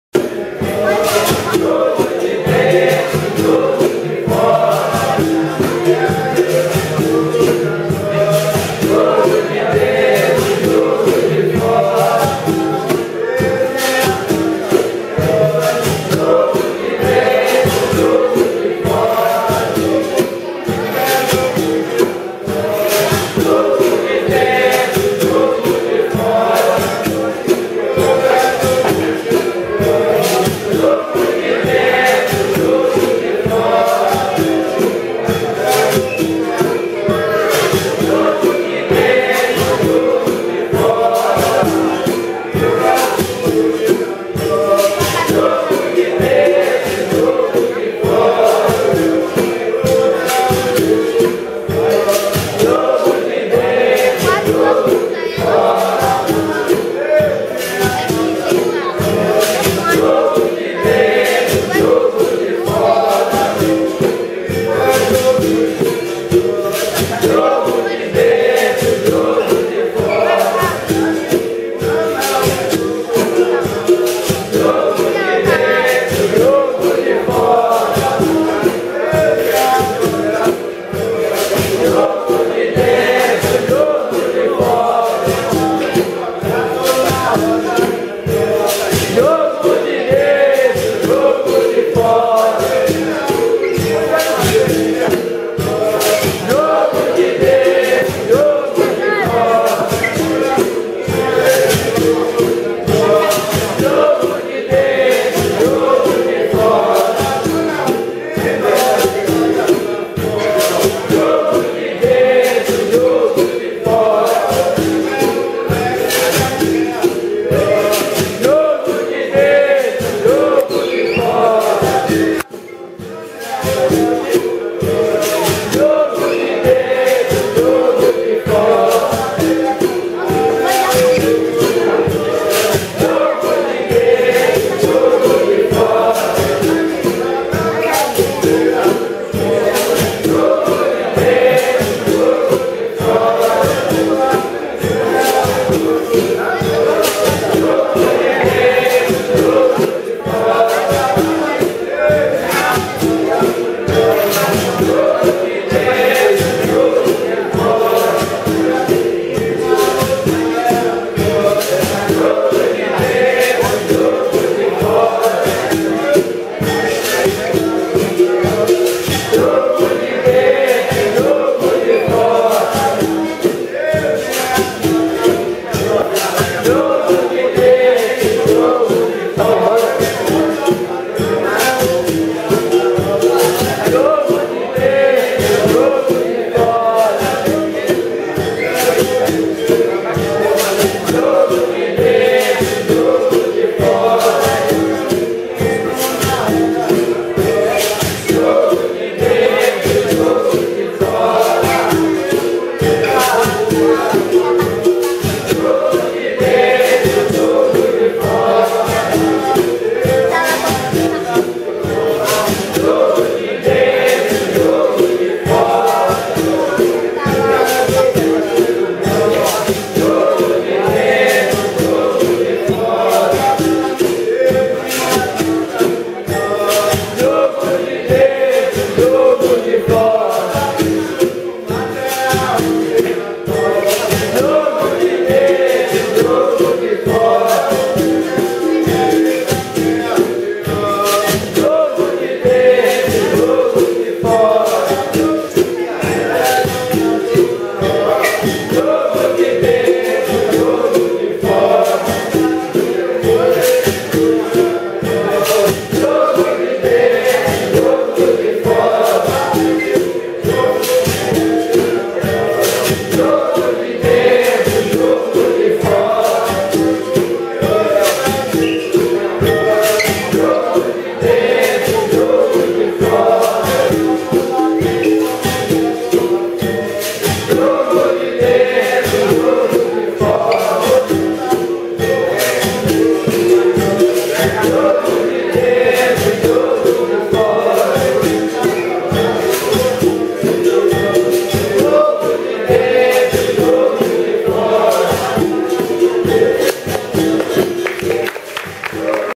Vadiação com Nani de João Pequeno - Evento "De Vento em Popa" - 2012
Vadiação com Nani de João Pequeno - Evento "De Vento em Popa" - 2012 em Mar Grande (Ilha de Itaparica) do Grupo de Capoeira Angola Paraguassú - Mestre Jaime de Mar Grande